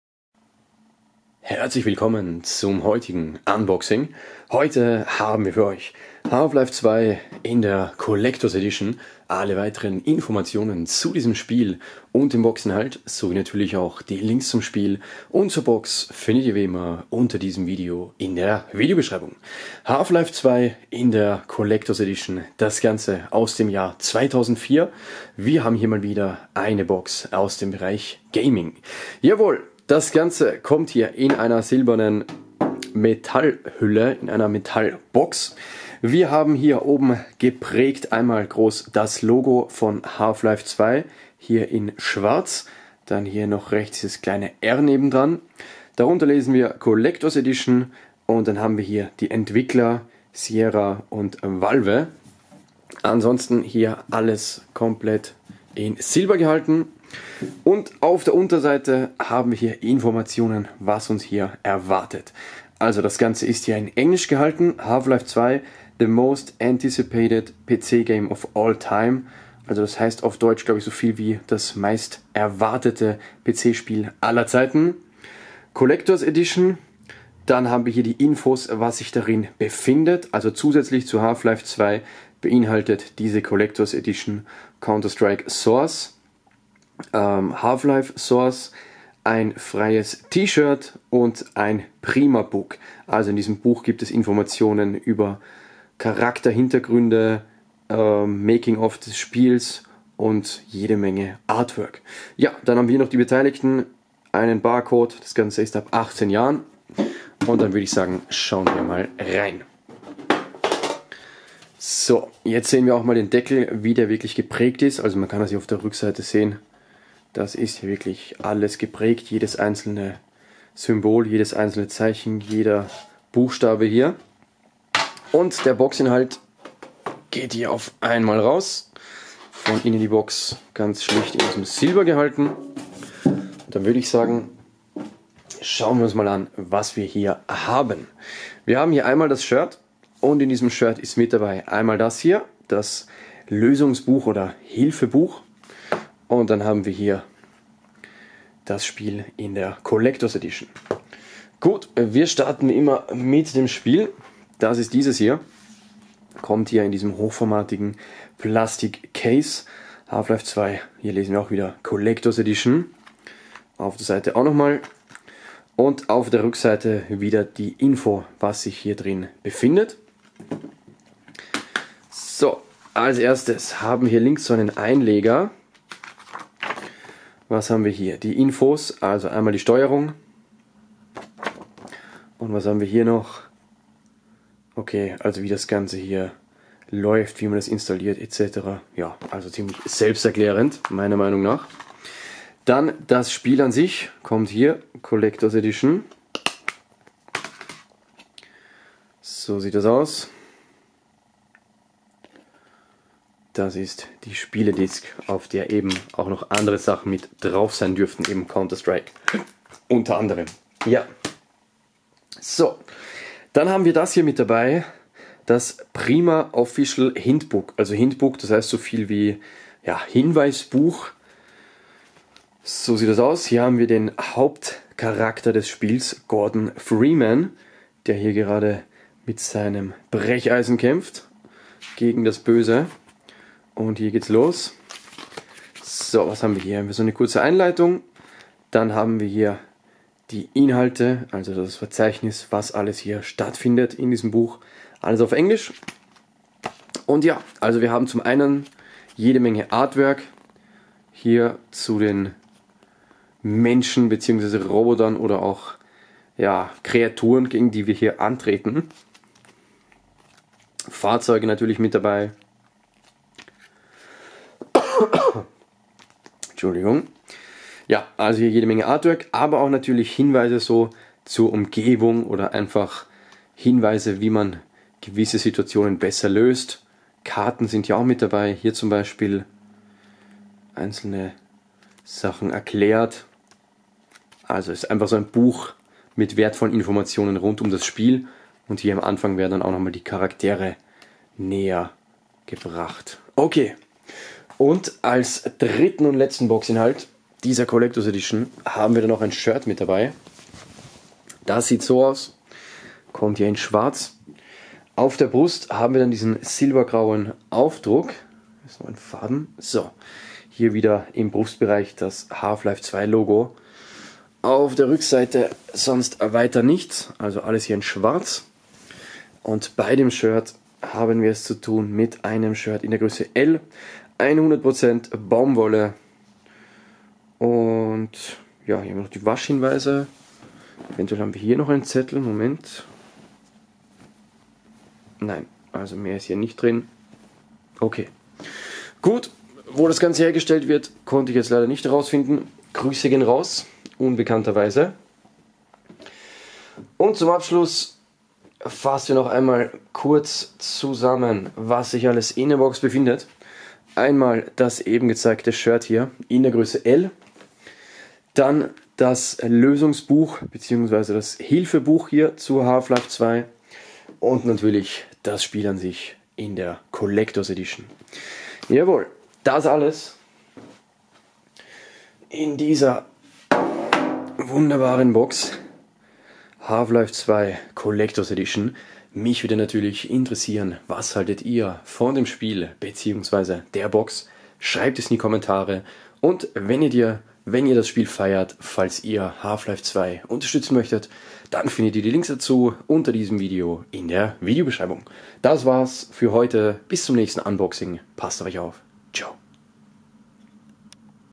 Half Life 2 Collectors Edition Unboxing German
In diesem Video zeige ich euch die Half Life 2 Collectors Edition.
► Hier könnt ihr das Spiel kaufen:
Box: ausverkauft
Weitere Spiele

✘► SPIEL INFOS
Kaufpreis: ? €

Half Life 2 Box Inhalt:
- Spiel
- T-Shirt
- Lösungsbuch

Half Life 2 Info:
Das Spiel erschien am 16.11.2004 über Valve.